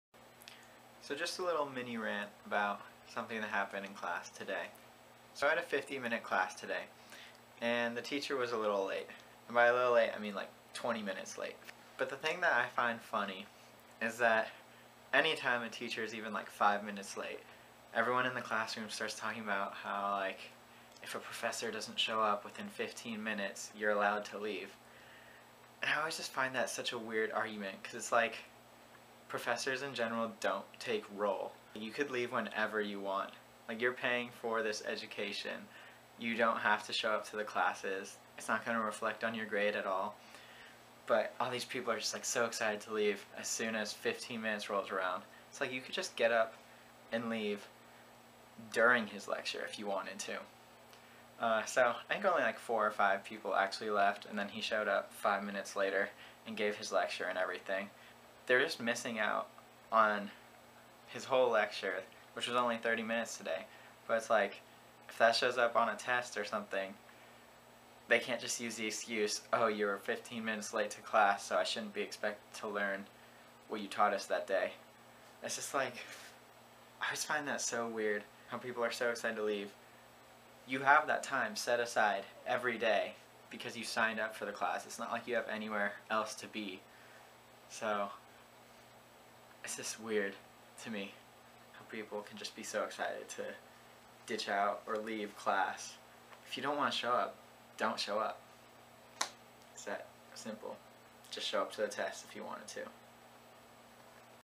Rant About Leaving Class Early
People in class are always so excited to leave their classes early.  They will always state some made up "fact" about how if a professor doesn't show up within 15 minuted of class then as students we are all allowed to leave.  This sounds pretty false to me so here is my little rant about it.  This just happened the other day to me